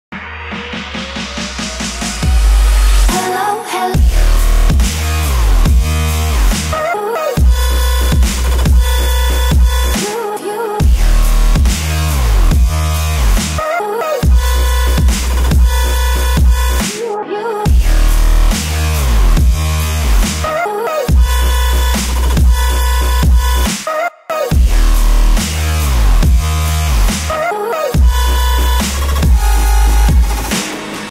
KPOP MIX || W I T H O U T / Y O U
Should I keep this up?
I'm really busy atm but I really badly felt like editing some dubstep soooo here it is :P sorry if its bad, I was feeling stressed since I was kind of procrastinating stuff I needed to do by doing this..